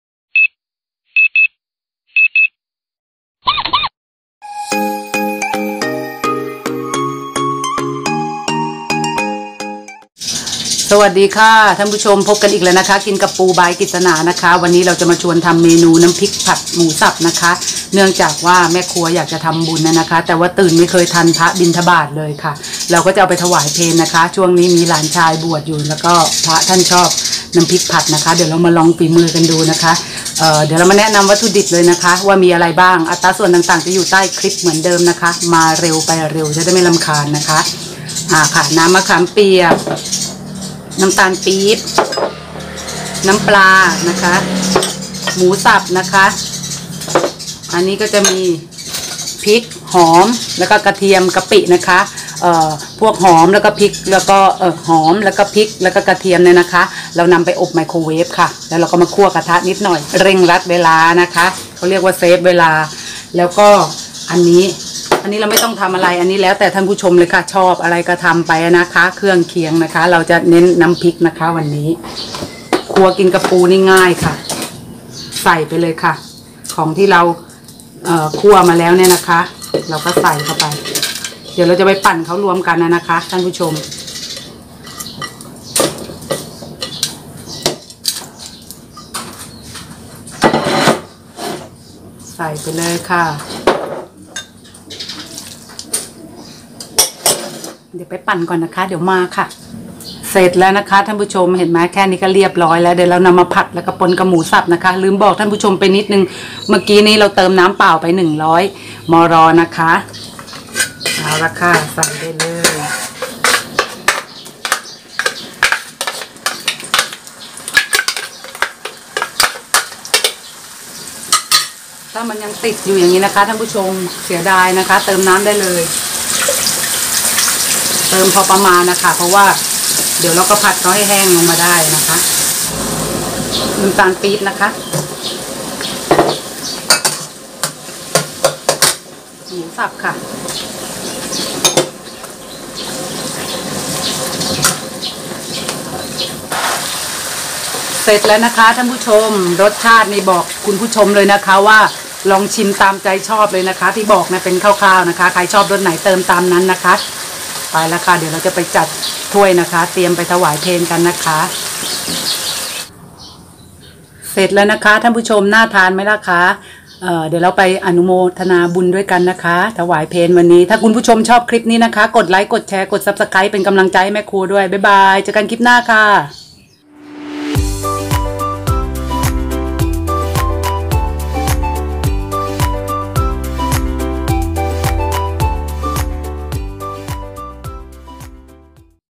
แจกสูตรวิธีทำน้ำพริกเผาผัดหมูสับกินเองง่ายๆแต่อร่อย#กินกับปู
เครื่องปรุงวัตถุดิบ
หอมแดง             120 กรัม
พริกแก้ว                25 กรัม
หมูบด                  200 กรัม
กะปิ                          2 ชต.
น้ำมะขาม             170 กรัม
น้ำปลา                 100 กรัม